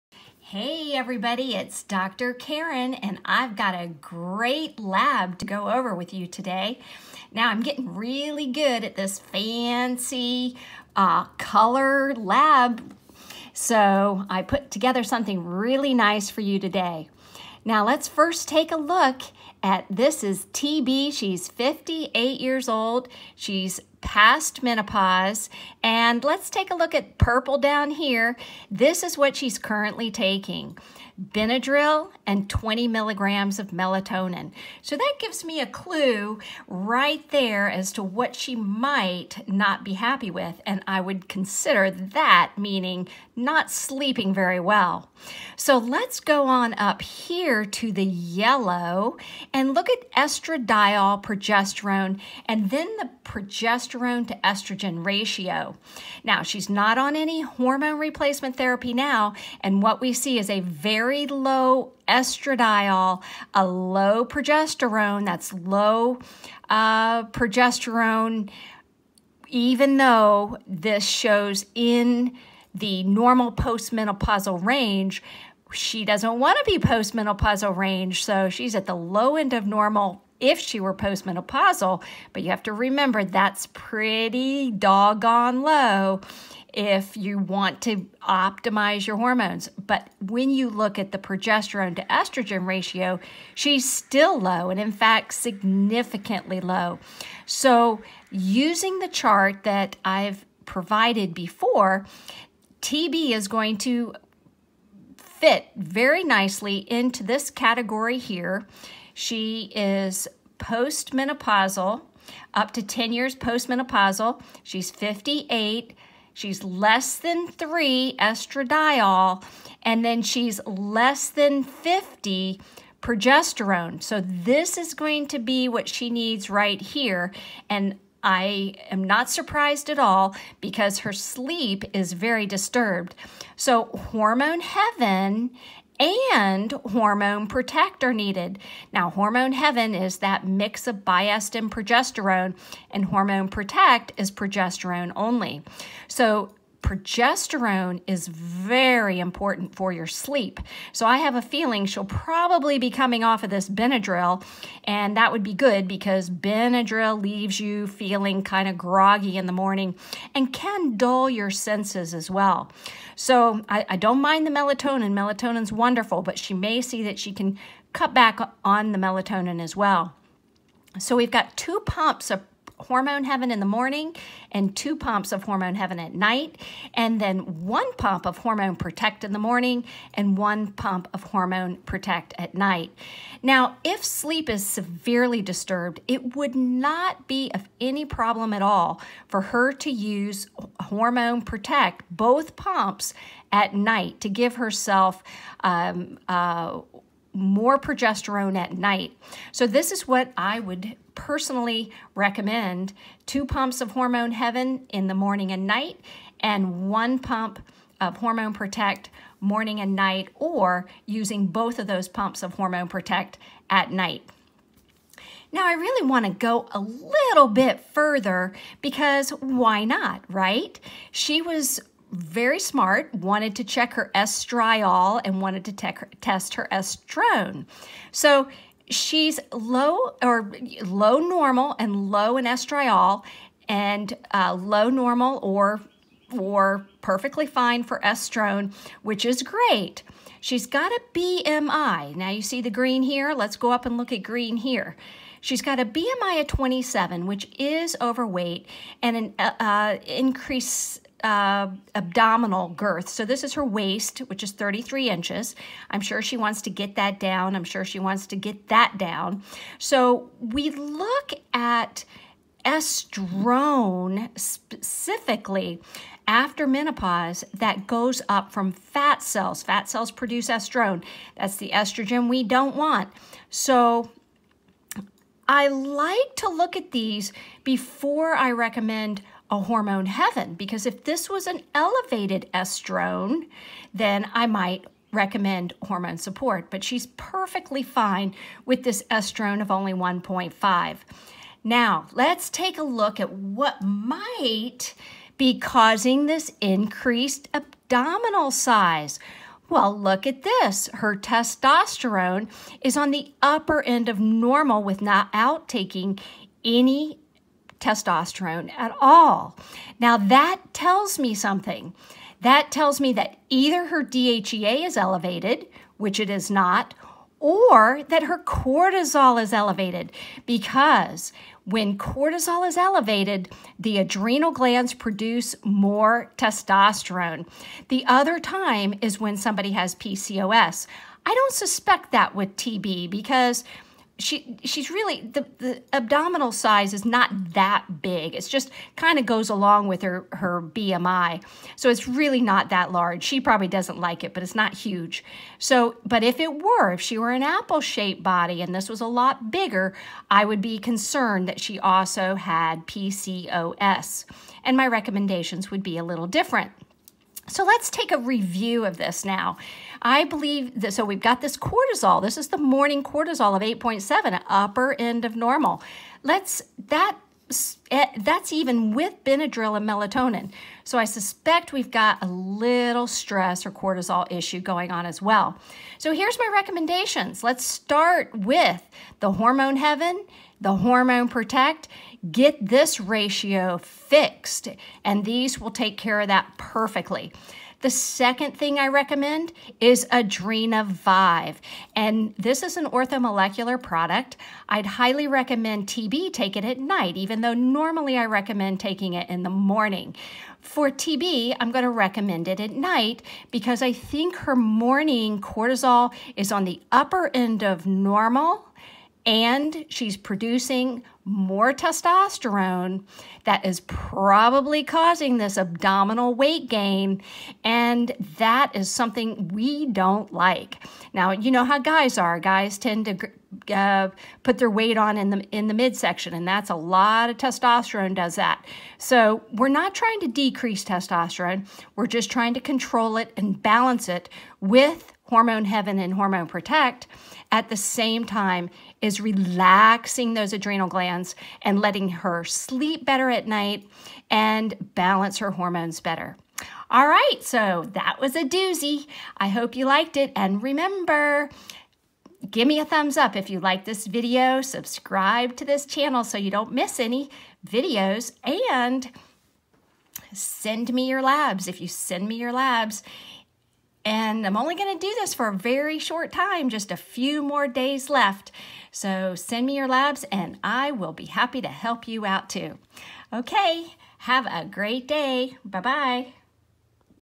Free Lab Review! WMS drkarenleggett.com
If you have gained weight and also have difficulty sleeping, You will want to watch this lab review!